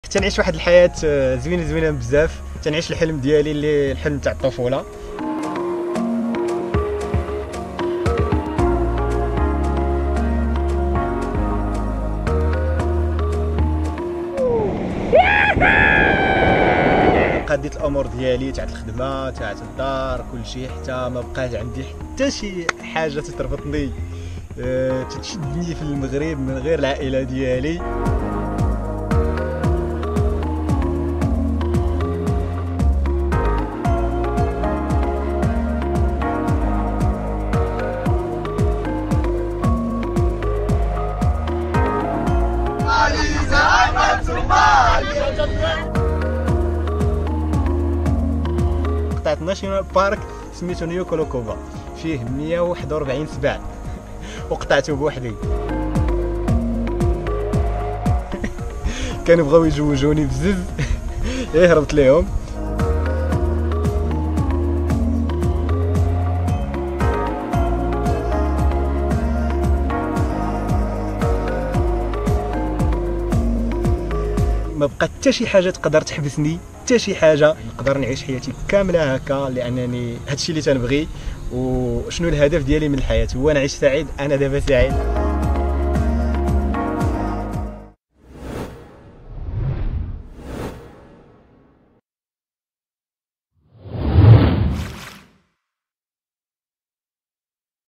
!مغربي يجول العالم
المغربي ياسين غلام يجول العالم على دراجة وبدون نقود! اكتشفوا المغامرات التي حكاها لواضح! 🚲🌍💪
تابعوه على :

Wada7 Original